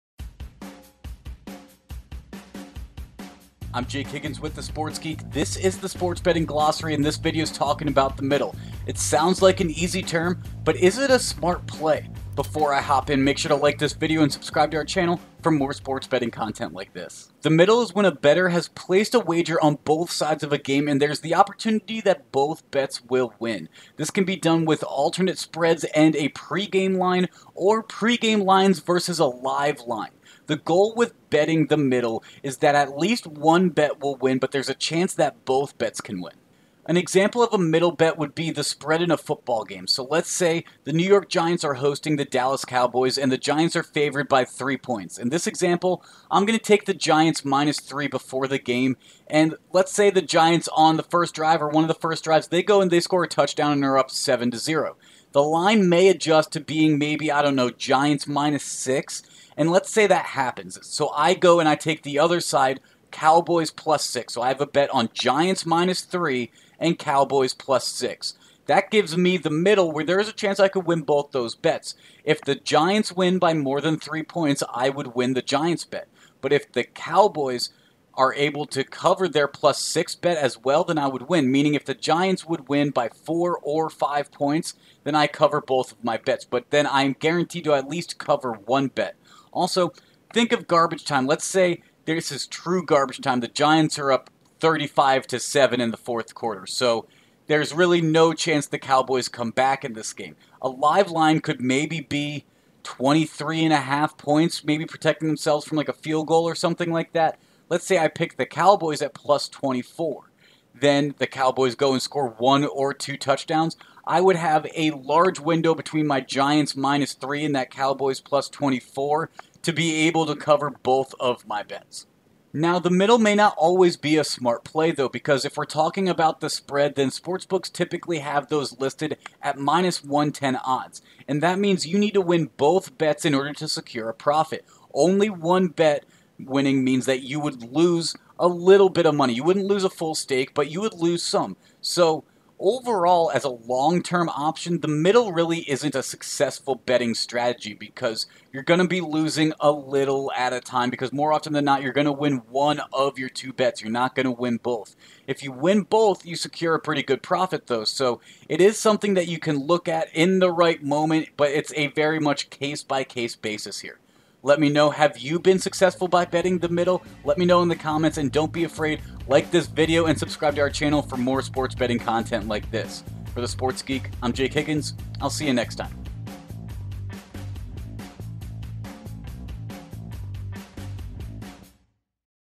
The Sports Geek's Sports Betting Glossary - The Middle
We're back with another sports betting terms video. Today we're talking about The Middle. This is a bet you can place where there is a possibility of winning 2 bets where scores overlap.